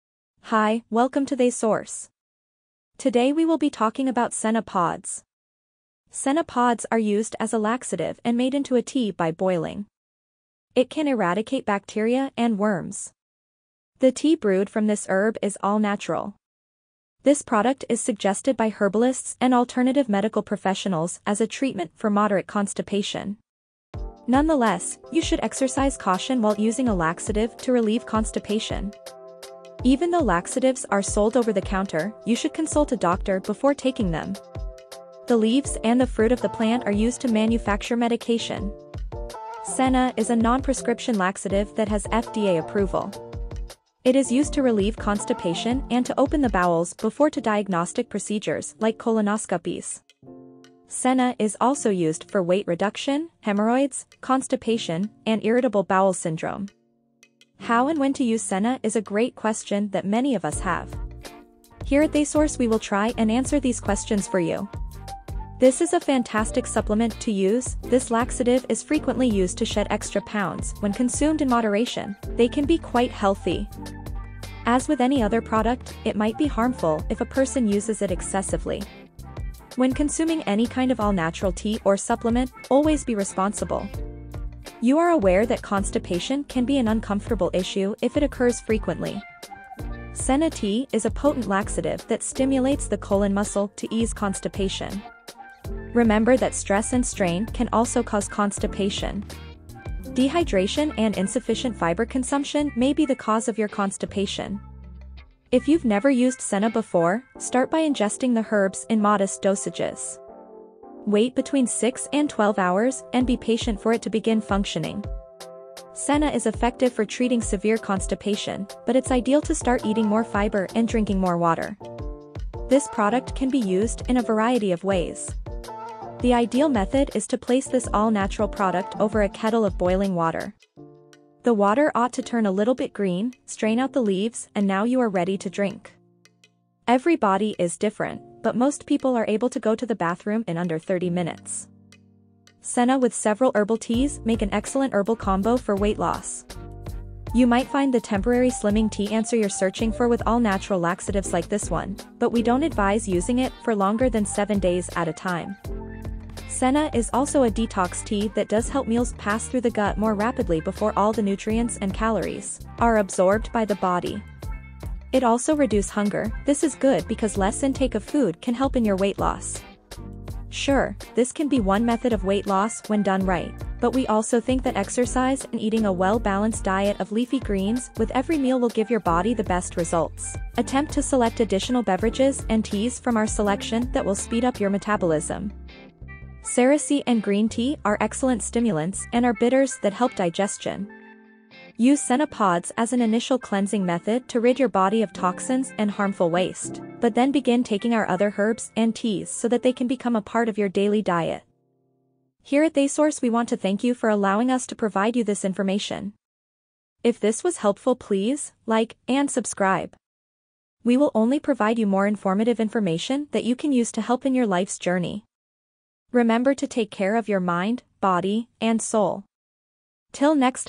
How To Use Senna Pods For Constipation Relief
We explain what senna pods are and how to use senna pods for constipation relief. Many people use this all-natural herb for different reasons. Weight loss and an all-natural laxative are just a couple.